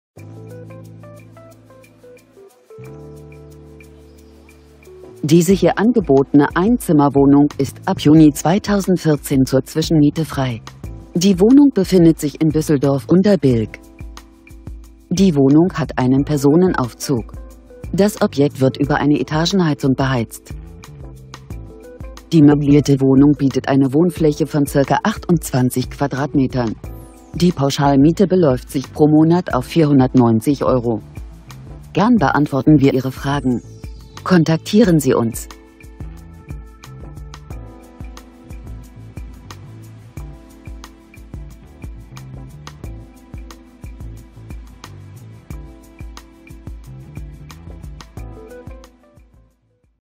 Medienhafen/Únterbilk: Funktionelles, helles Apartment in der Trendlage Hafen. Parkhaus in der
Preis: 490€
Zimmer: 1
Wohnfläche: ca. 28m²

 Mitwohnzentrale